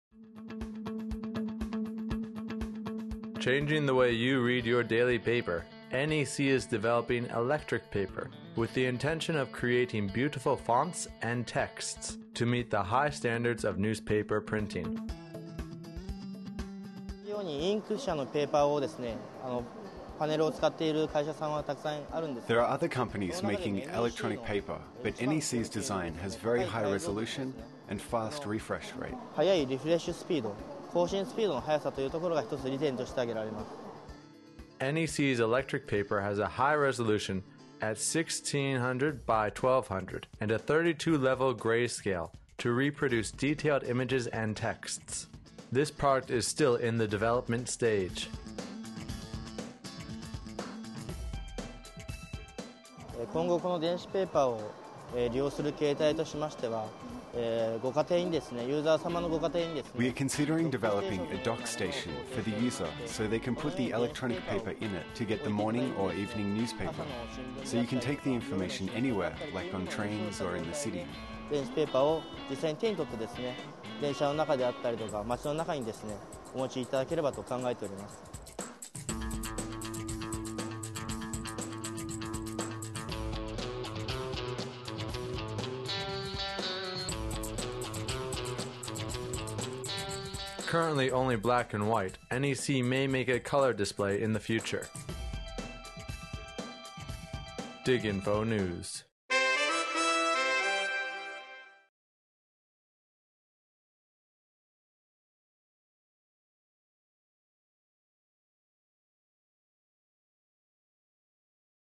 NEC's Electric Paper : DigInfo
Changing the way you read your daily paper, NEC is developing Electric Paper with the intention of creating beautiful fonts and texts to meet the high standards of newspaper printing.

NEC's electric paper has a high resolution at 1600x1200 and a 32 level gray scale to reproduce detailed images and texts.  This product is still in the development stage.

Currently only black and white, NEC may make a color display in the future.